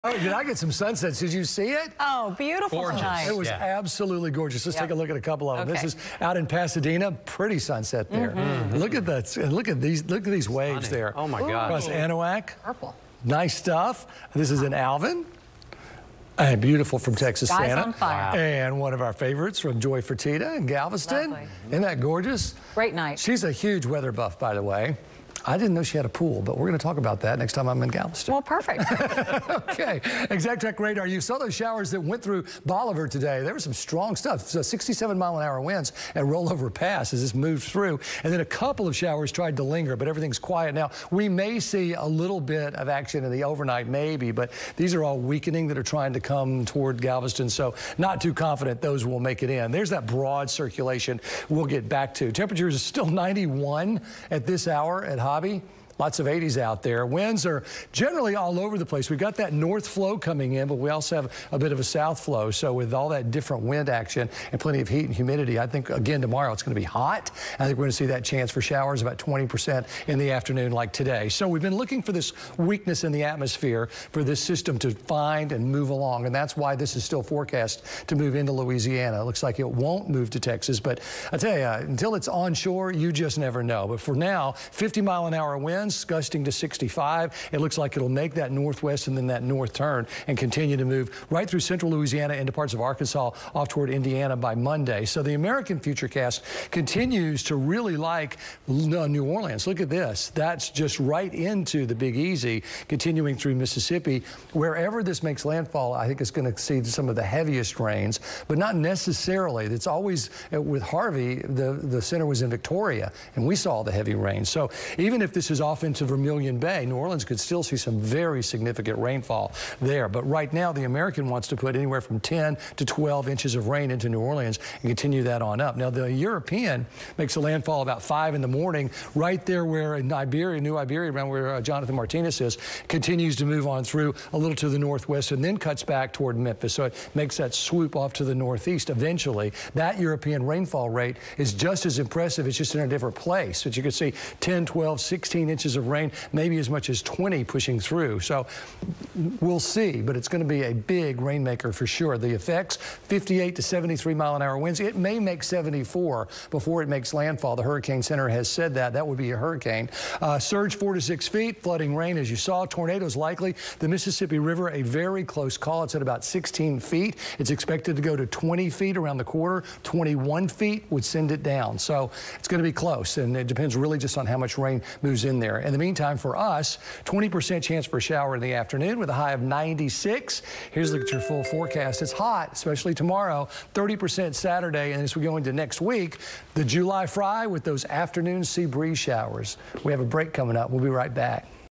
Tracking the tropics: Frank's 10pm update July 11, 2019
Tropical Storm Barry formed Thursday morning in the Gulf of Mexico.